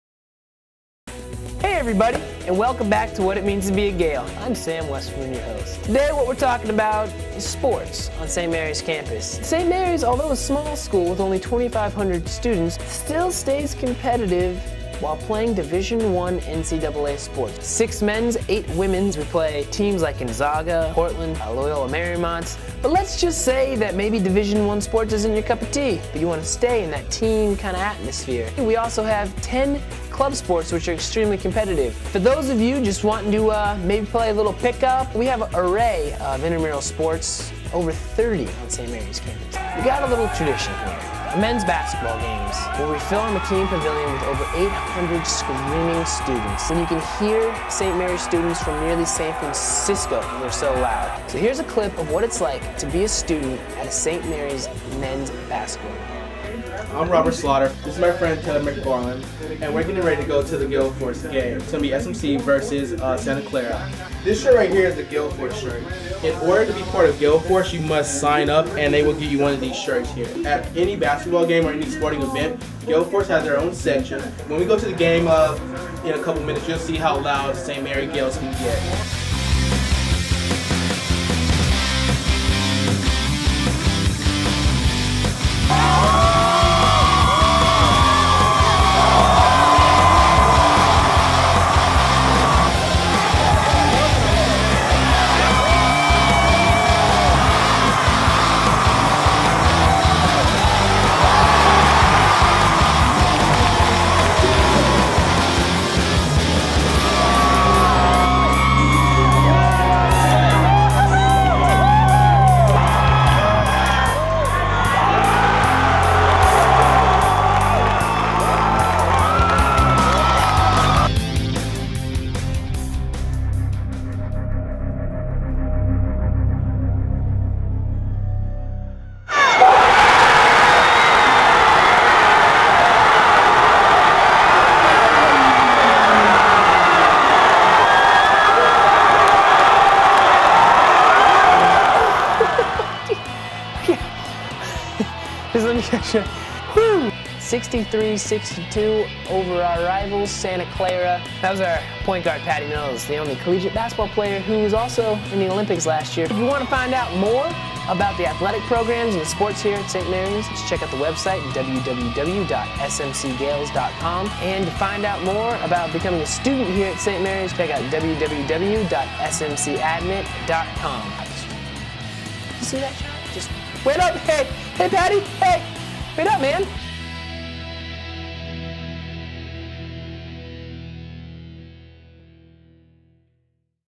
What it Means to be a Gael: GaelForce
Being a student at Saint Mary's also means being a fan of Saint Mary's. Get a sneak peak of what it means to be a Gael and a member of GaelForce